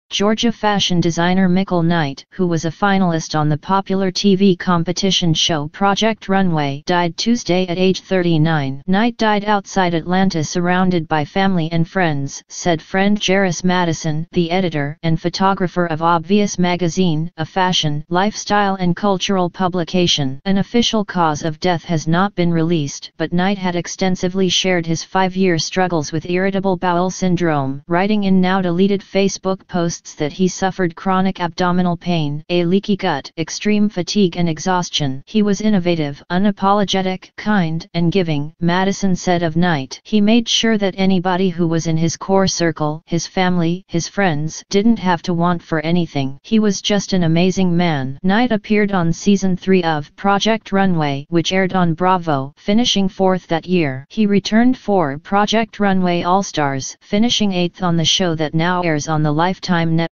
Mychael Knight, ‘Project Runway’ Fashion Designer, Dies at 39
Georgia fashion designer Mychael Knight, who was a finalist on the popular TV competition show "Project Runway," died Tuesday at age 39.

Knight died outside Atlanta surrounded by family and friends, said friend Jerris Madison, the editor and photographer of Obvious Magazine, a fashion, lifestyle and cultural publication.

An official cause of death has not been released, but Knight had extensively shared his five-year struggles with irritable bowel syndrome, writing in now-deleted Facebook posts that he suffered chronic abdominal pain, a "leaky gut," extreme fatigue and exhaustion.